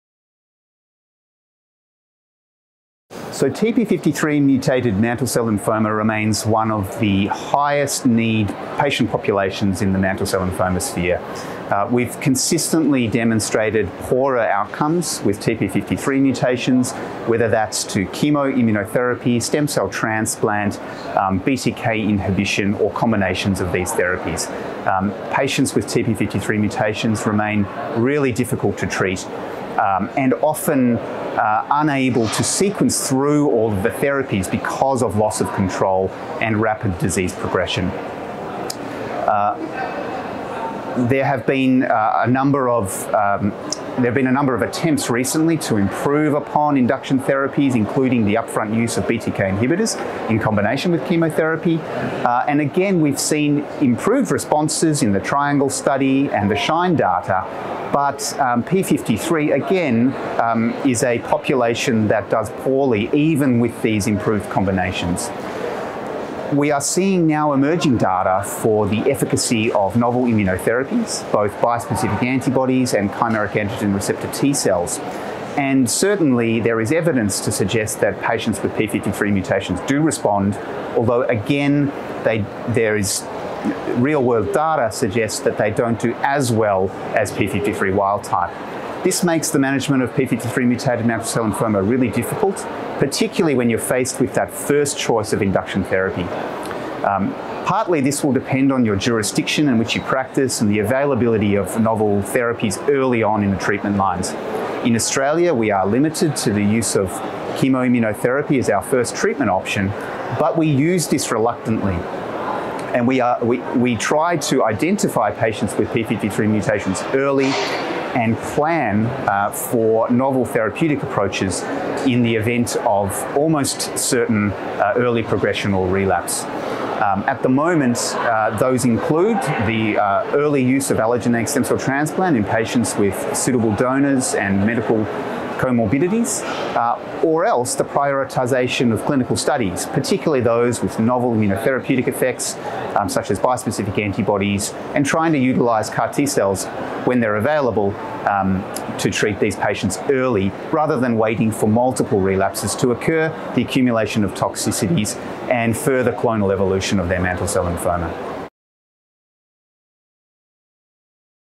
Challenges in treating patients with TP53-mutated MCL & promising novel therapies being explored
Adrian Minson, MBBS, Peter MacCallum Cancer Centre and Royal Melbourne Hospital, Melbourne, Australia, discusses challenges in treating patients with TP53-mutated mantle cell lymphoma (MCL), emphasizing the poor outcomes demonstrated with current treatment approaches including chemoimmunotherapy (CIT), stem cell transplant (SCT), and Bruton's tyrosine kinase (BTK) inhibitors. Dr Minson further comments on strategies being explored to improve outcomes for patients, and discusses the promise of novel immunotherapies, including bispecific antibodies and CAR-T cells. This interview took place at the 28th Congress of the European Hematology Association (EHA) 2023 in Frankfurt, Germany.